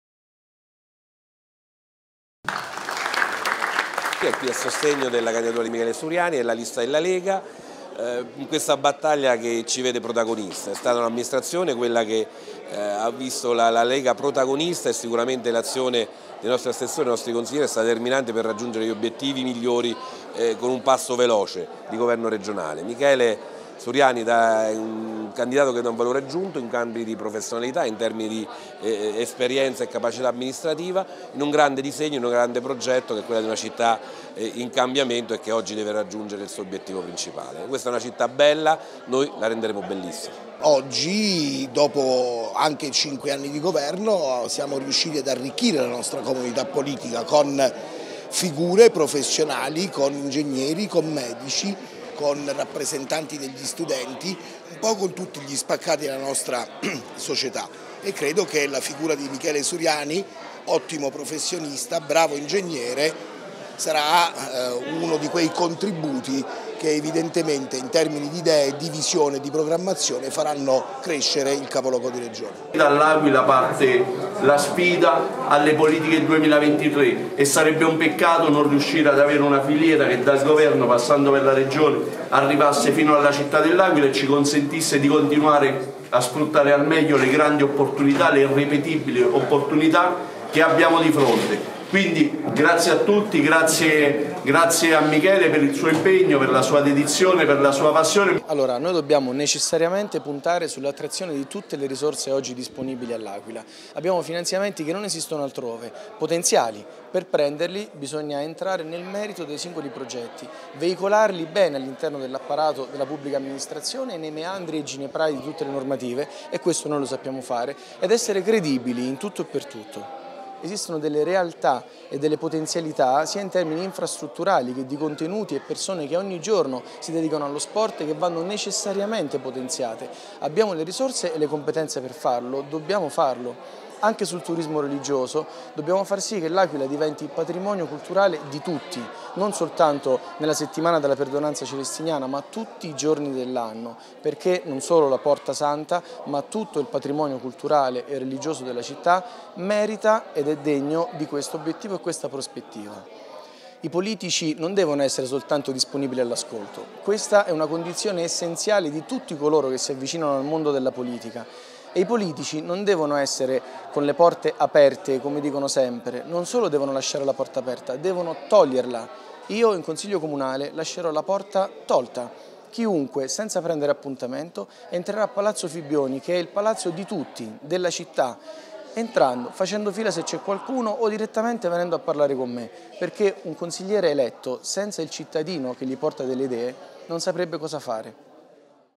VOTO L'AQUILA: SURIANI CONCLUDE CAMPAGNA ELETTORALE, "COMPETENZA E VISIONE PER FONDI PNRR"
L'AQUILA -  Nei prossimo anni  dobbiamo necessariamente puntare sul migliore utilizzo di tutte le risorse oggi disponibili a L'Aquila: abbiamo ingenti finanziamenti potenziali, ma per prenderli, per creare lavoro buono e stabile, bisogna entrare nel merito dei singoli progetti, veicolarli bene all'interno dell'apparato della pubblica amministrazione, essere credibili in tutto e per tutto, superare i gineprai delle normative, e questo noi lo sappiamo fare".

Davanti ad oltre 200 persone, all'Auditorium Ance dell'Aquila,  il 42enne Michele Suriani, ingegnere candidato nella lista della Lega alle elezioni comunali dell'Aquila del 12 giugno prossimo, si è presentato come un tecnico che ha deciso di mettere a disposzione le sue competenze di funzionario del Comune dell'Aquila e capo del Dipartimento Infrastrutture e Pnrr abruzzese del Carroccio.

All’evento promosso da Suriani, hanno partecipato il segretario regionale della Lega Abruzzo, il deputato Luigi D'Eramo, il vice presidente della Giunta regionale, Emanuele Imprudente, e il candidato sindaco del Centrodestra, Pierluigi Biondi.

Entrando le merito, Suriani ha ricordato che "con il Pnrr ordinario e quello complementare dedicato ai cateri sismici 2009 e 2016,  si possono sviluppare importanti progetti nell'ambito della  cultura, e del sociale e dello sport, sia in termini infrastrutturali che di contenuti e persone che ogni giorno si dedicano a questi ambiti. In particolare importante puntare sul turismo religioso: dobbiamo far sì che la Perdonanza celestiniana diventi patrimonio culturale e volano turistico tutti i giorni dell'anno, e non solo durante la settimana dell'apertura della porta santa".

Nel suo intervento D'Eramo ha evidenziato che  la Lega ha saputo comporre una lista che è formata in maniera assolutamente equilibrata da militanti, da persone che hanno costruito questo partito in questi sette anni, e da figure professionali, come ingegneri, medici, rappresentanti degli studenti, professionisti nei vari ambiti della nostra società. Ebbene,  credo che è la figura di Michele sia quella di un ottimo professionista, un bravo ingegnere  e darà un contributo  prezioso in termini di idee, visione e programmazione per faranno crescere ancora il capoluogo di regione".

Concorda Imprudente: " Suriani è un esempio di grande professionalità, esperienza e capacità amministrativa, una delle persone che servono per far camminare il nostro disegno di un grande progetto della città in cambiamento. Questa è una città bella, noi nei prossimi 5 anni la renderemo bellissima".

Ha concluso Suriani: "i politici in campagna elettorale promettono che la loro porta rimarrà sempre aperta. Io arriverò se eletto a toglierla proprio e  chiunque senza prendere appuntamento potrà entrare nel palazzo di Città, che è il palazzo di tutti i cittadini ad incontrarmi, a segnalare problemi, proporre soluzioni, illustrare buone idee".